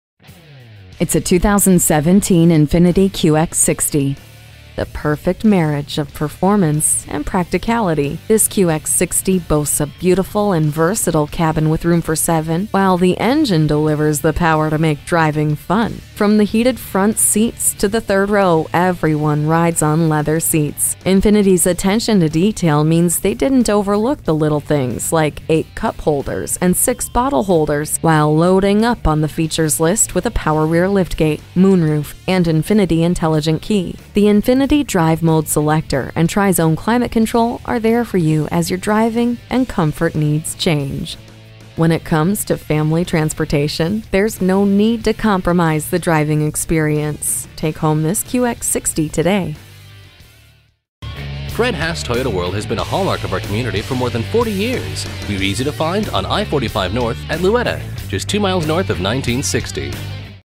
Used 2017 INFINITI QX60 Spring TX, TX HC549947P - SOLD
Year: 2017
Make: INFINITI
Model: QX60
Engine: 6 Cyl. 3.5 L
Transmission: Variable
Mileage: 13648
Stock #: HC549947P
VIN: 5N1DL0MN4HC549947
Scores 27 Highway MPG and 20 City MPG! Carfax One-Owner Vehicle. This INFINITI QX60 boasts a Premium Unleaded V-6 3.5 L/213 engine powering this Variable transmission. Wireless Streaming, Window Grid And Roof Mount Diversity Antenna, Wheels: 18 Triple 5-Spoke Aluminum-Alloy.*This INFINITI QX60 Features the Following Options *Wheels w/Silver Accents, -Way Passenger Seat, -Way Driver Seat -inc: Manual Lumbar Support, Valet Function, Trunk/Hatch Auto-Latch, Trip Computer, Transmission: Sport-Tuned CVT -inc: manual shift mode, Transmission w/Driver Selectable Mode and Oil Cooler, Tires: P235/65R18 AS T-Rated, Tire Specific Low Tire Pressure Warning.*Stop By Today *A short visit to Fred Haas Toyota World located at 20400 Interstate 45, Spring, TX 77373 can get you a trustworthy QX60 today!

Address:
20400 Interstate 45 North